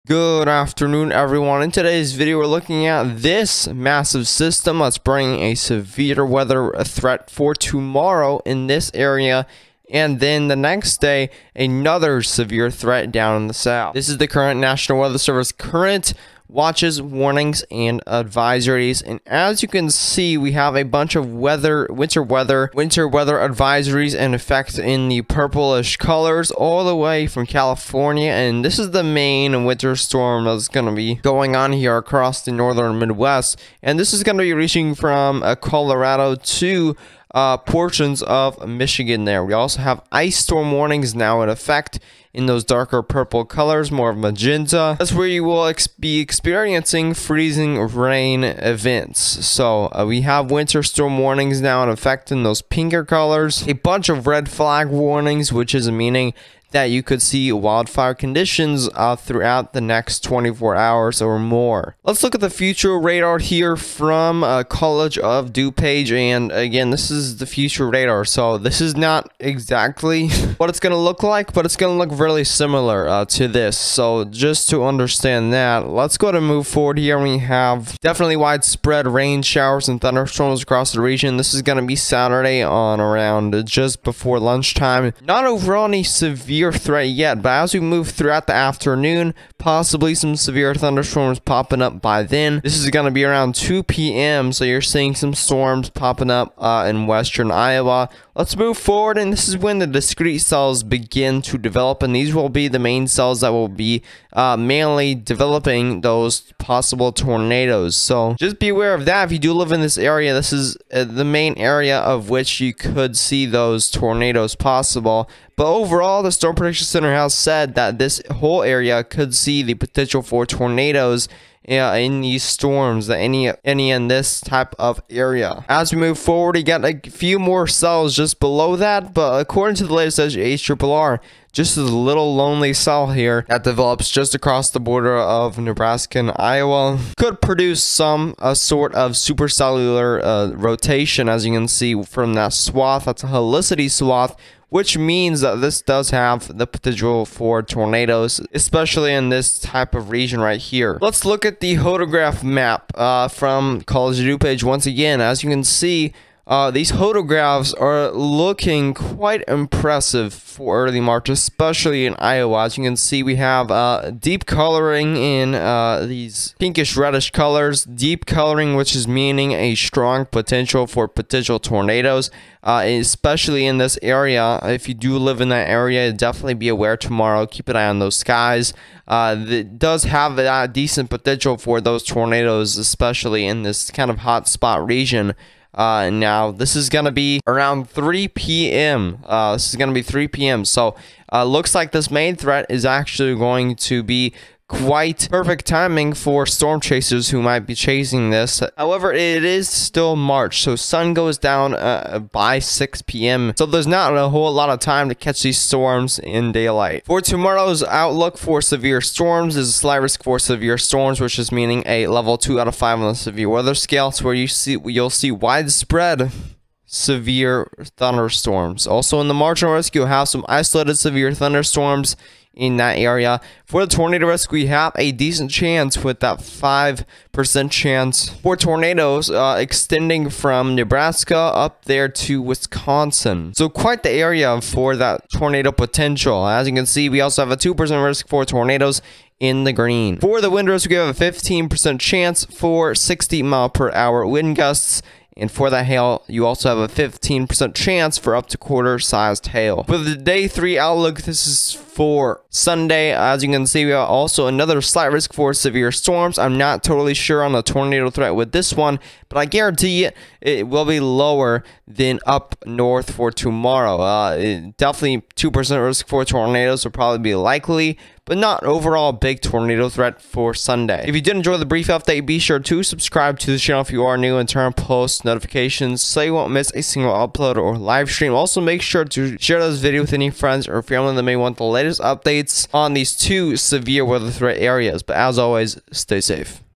This Weekend's BIG Storm Bringing a Snowstorm, Ice Storm, & Tornado Threat
In today's video we're going over the big weekend storm system to bring a Snowstorm to the midwest, following with an ice storm, and tornado threats at the same time!

Intro: 0:00
Current Hazards: 0:14
Future Radar For Tomorrow: 1:01
Tornado Models: 2:31
Severe Thunderstorm Outlook: 3:27

THANK YOU TO OUR CHANNEL MEMBERS:
(EF4) Caleb Downey
(EF2) ThemePro
(EF2) Weather Express
(EF2) IL SoundOp